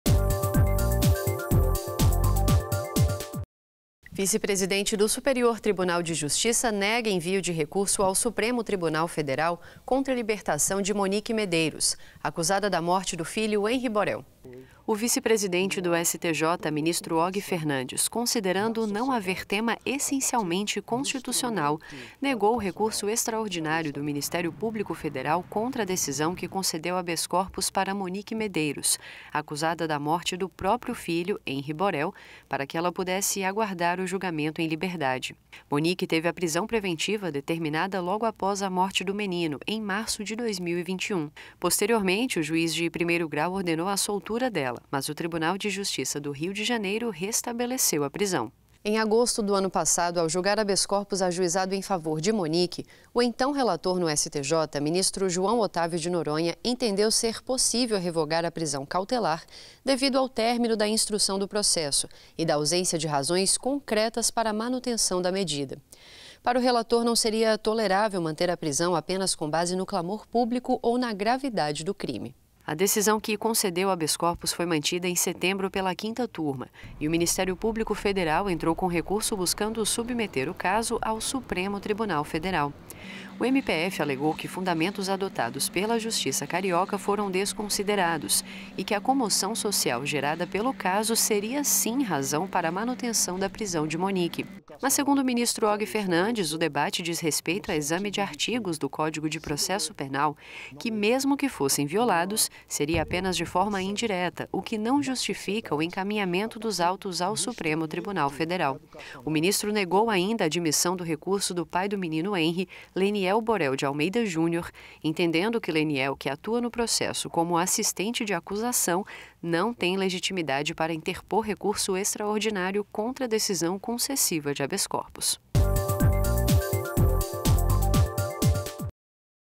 Vice-presidente do STJ nega envio ao STF de recurso contra libertação de Monique Medeiros 28.03.23
Por não verificar a existência de tema essencialmente constitucional, o vice-presidente do Superior Tribunal de Justiça (STJ), ministro Og Fernandes, inadmitiu o recurso extraordinário do Ministério Público Federal (MPF) contra a decisão que concedeu habeas corpus a Monique Medeiros, acusada da morte de seu filho, Henry Borel, para que ela pudesse aguardar o julgamento em liberdade.